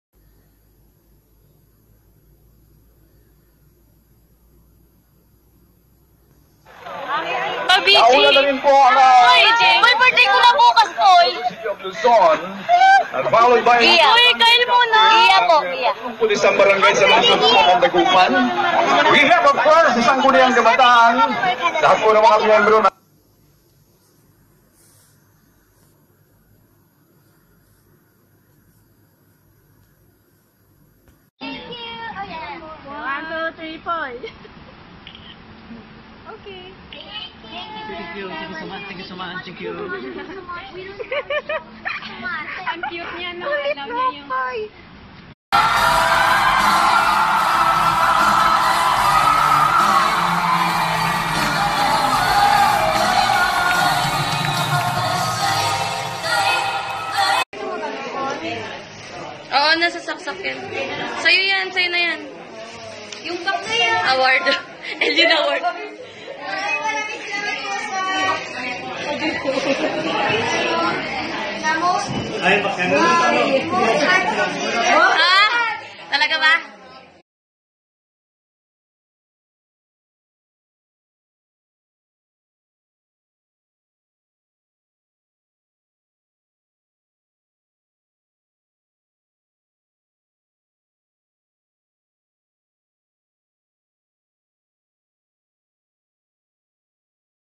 KEIFER SANCHEZ (TNT BOYS)Random Videos
I can't help but adore him as he is. He always does things that make us fall in love with him...Associating with his fans with so much affection and compassion are beyond words. While on a motorcade parade, a loving fan asked for a bday greeting and with a smile he just complied...Isn't he the sweetest idol? As i get to know him more he gains my respect and adoration much deeper. I can proudly say I AM A SOLID AND FOREVER FAN OF THIS AMAZING, ADORABLE YOUNG BOY. PLEASE REMAIN WONDERFUL BABY POY. WE LOVE YOU JUST THE WAY YOU ARE.
" IMPRESSIVE, VIBRANT PERSONALITY-KEIFER SANCHEZ ☆☆☆

Video Credit KEIFER BELIEVERS FAM